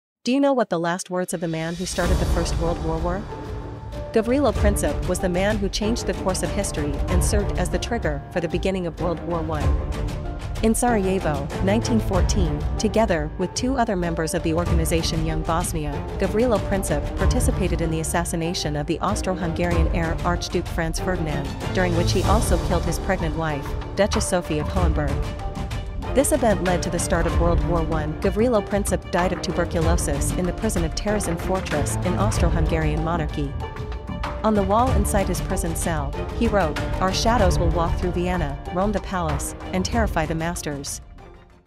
The Final Words of the Man who Started World War I
The last words that Gavrilo Princip wrote on the wall of his prison cell, before he died.